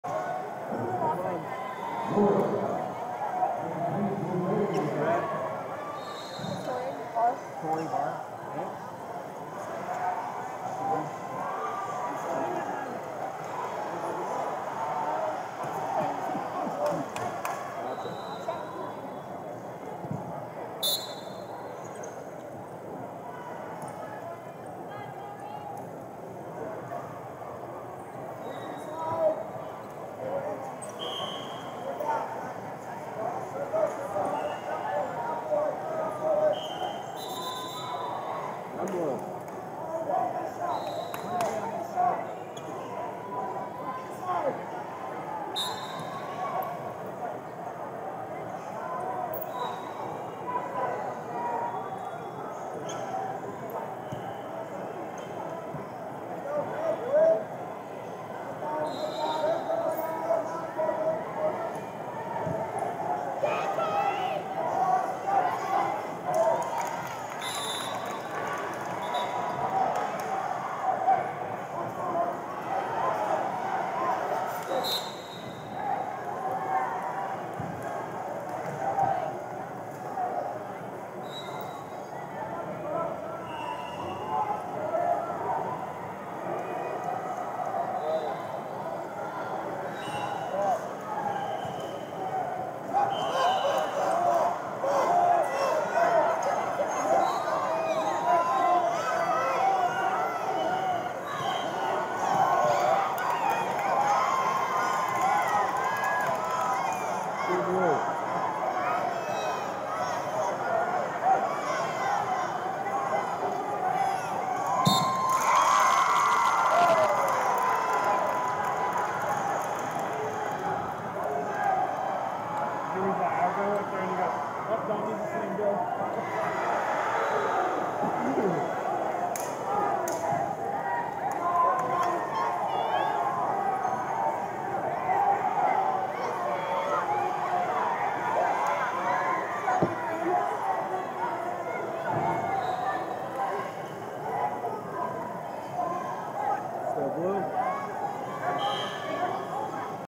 132 Lbs Placement Matches (8 Team) - Tori Bahr, Minnesota Red Vs Shelbie Brehm, South Dakota Blue
Flosports-partner-access/2023 Junior National Duals And 14U Girls Duals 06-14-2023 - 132 Lbs Placement Matches (8 Team) - Tori Bahr, Minnesota Red Vs Shelbie Brehm, South Dakota Blue 509e